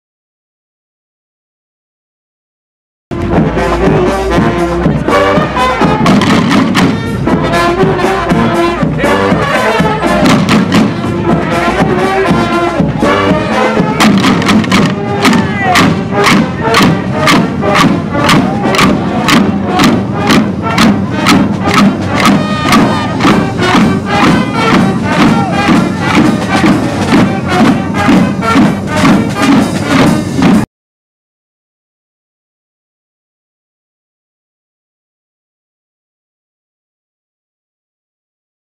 Ikasbide eta Morlanstarrak danborradak elkarrekin
Ilundu orduko azken txanpan sartu da San Sebastian eguna. Aipamen berezia merezi dute Ikasbide eta Morlanstarrak danborradek. Elkarrekin aritu dira Amara Plaza hotelaren aurrean, giro paregabean.